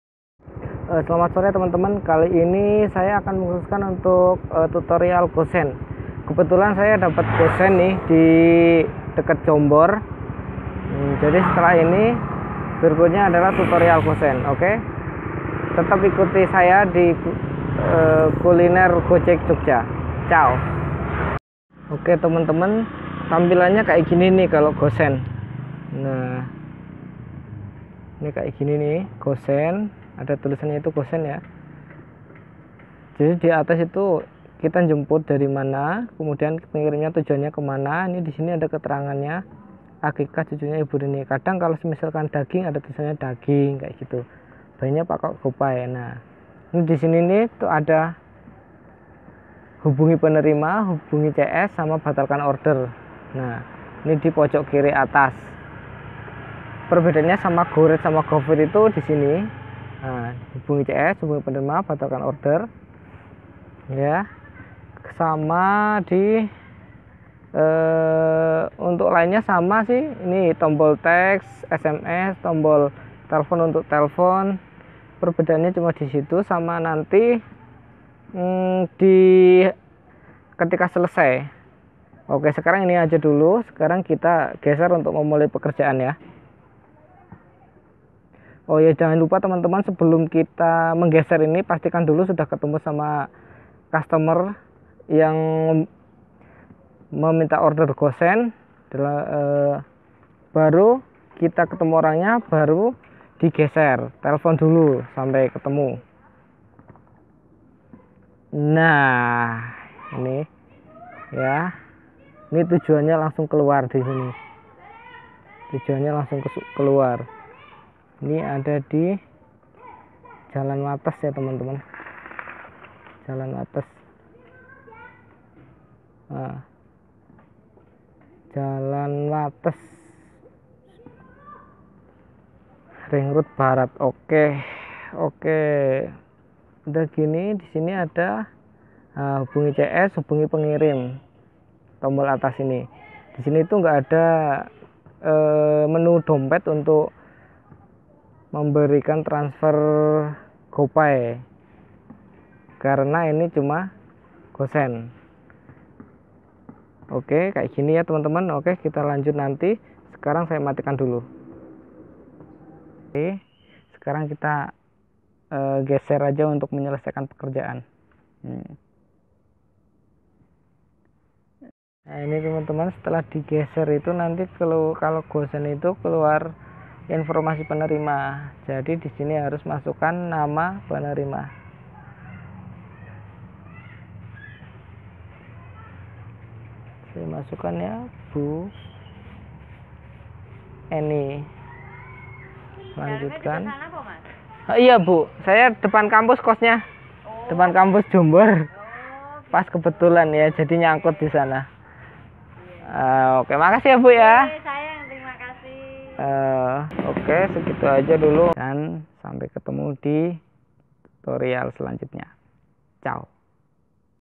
Tutorial Driver Gojek part 4 - Go Send
Berikut Contoh Tutorial Aplikasi Driver Gojek cara menerima Order Go-Send, dan Panduan untuk  driver gojek pemula dalam menerima Order Go-Send. Maaf Kalo videonya masih goyang-goyang.
Ayo Kita langsung cek saja di videonya.